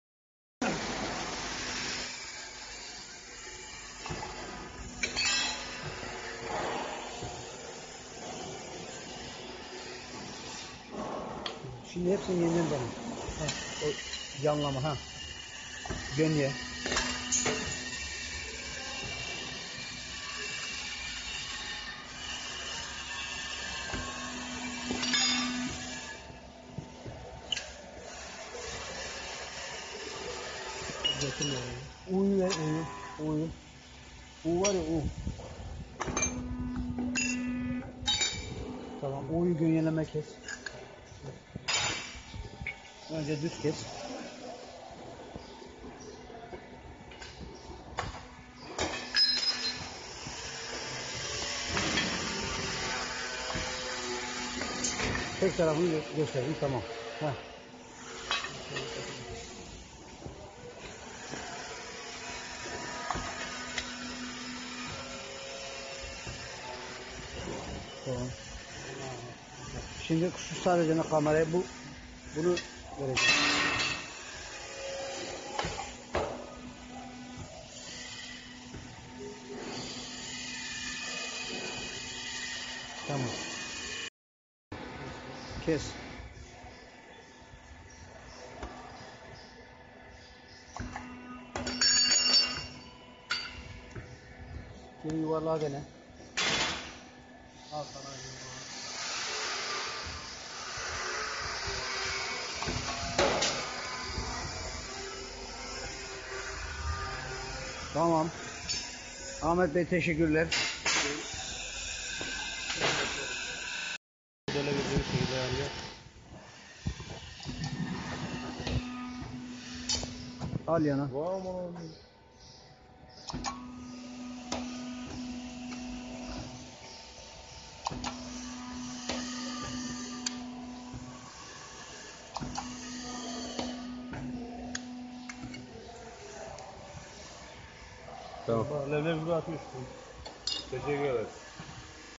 Hydraulic iron workers machine 💥
Kombine Shears

00:00 The hydraulic iron worker machine is starting to operate and is cutting the part.
01:27 Our triangular part is being cut on the Hydraulic Iron Workers machine.
02:13 Circular holes are being drilled into the part.